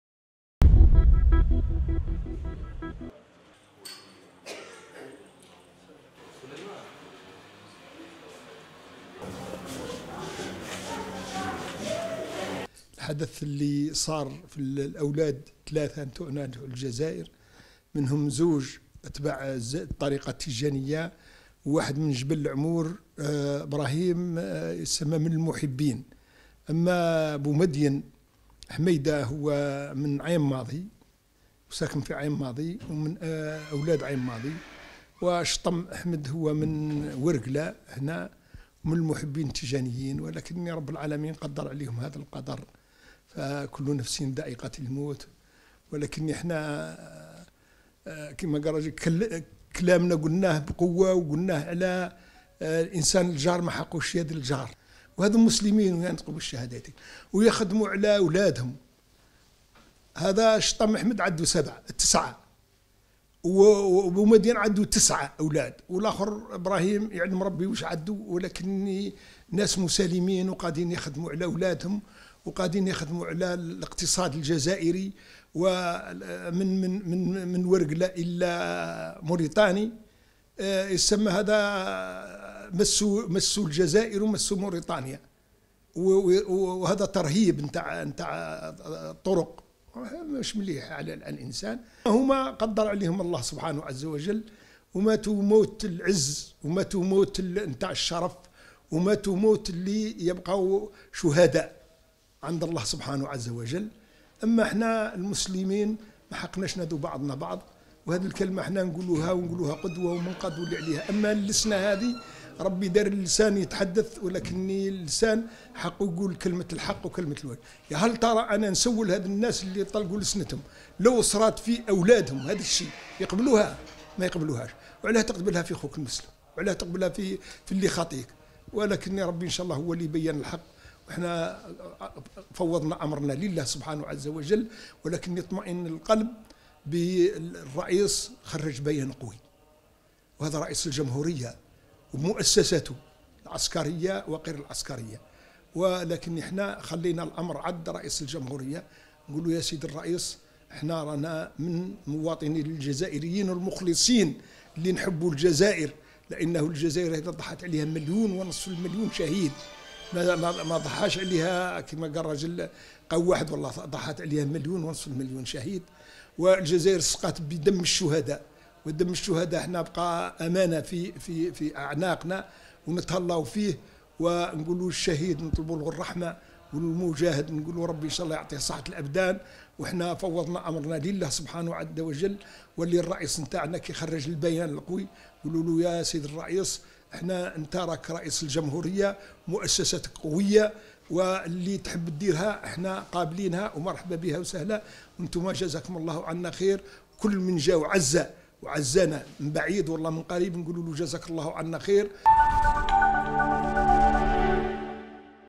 ورقلة: الخليفة العام للطريقة التيجانية يعزي عائلة الشهيد اشطم أحمد ضحية الاعتداء المغربي الغادر
ورقلة (وأج)- تنقل الخليفة العام للطريقة التيجانية سيدي علي بلعرابي، الى مسكن الشهيد اشطم أحمد، أحد ضحايا الاعتداء المغربي الغادر، لتقديم واجب العزاء لعائلة المرحوم، اين عبر في تصريح لـ (وأج) ثقته عن بالغ حسرته واساه جراء هذه الفاجعة الاليمة التي راح ضحيتها ثلاثة رعايا جزائريين ليلة الفاتح من نوفمبر المبارك.